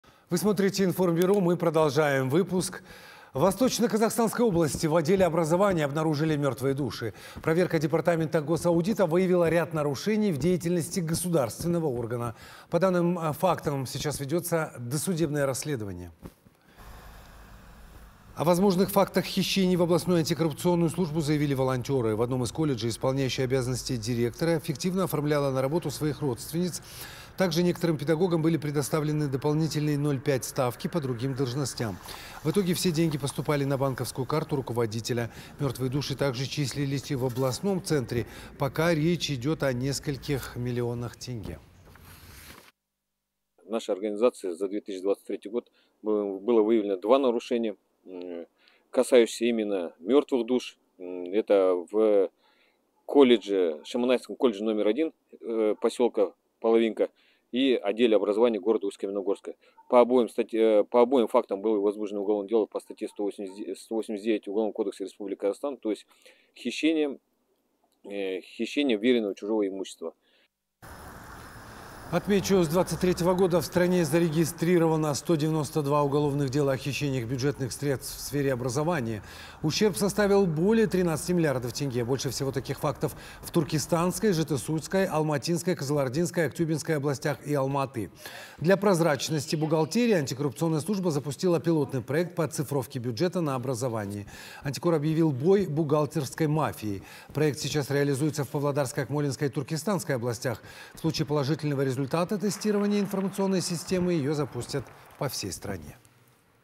В отделе образования ВКО обнаружили мертвые души
В Восточно-Казахстанской области в отделе образования обнаружили мертвые души. Проверка департамента госаудита выявила ряд нарушений в деятельности государственного органа. По данным фактам ведется досудебное расследование.
О возможных фактах хищений в областную антикоррупционную службу заявили волонтеры. В одном из колледжей исполняющий обязанности директора фиктивно оформляла на работу своих родственниц, также некоторым педагогам были предоставлены дополнительные 0,5 ставки по другим должностям. В итоге, все деньги поступали на банковскую карту руководителя. Мертвые души также числились и в областном центре. Пока речь идет о нескольких миллионах тенге.
АДИЛЬ ПШЕНОВ, руководитель филиала РОО "Әділдік жолы":
— Нашей организацией за 2023 год было выявлено два нарушения, касающихся именно мертвых душ. Это в Шемонаихинском колледже №1 поселка Половинка и отделе образования города Усть-Каменогорска. По обоим фактам было возбуждено уголовное дело по статье 189 УК РК — то есть, хищение вверенного чужого имущества.
С 2023 года в стране зарегистрировано 192 уголовных дела о хищениях бюджетных средств в сфере образования. Ущерб составил более 13 млрд тенге. Больше всего таких фактов в Туркестанской, Жетысуской, Алматинской, Кызылординской, Актюбинской областях и Алматы. Для прозрачности бухгалтерии антикоррупционная служба запустила пилотный проект по оцифровке бюджета на образование. Антикор объявил бой «бухгалтерской мафии». Проект сейчас реализуется в Павлодарской, Акмолинской и Туркестанской областях. В случае положительного результата тестирования информационной системы ее запустят по всей стране.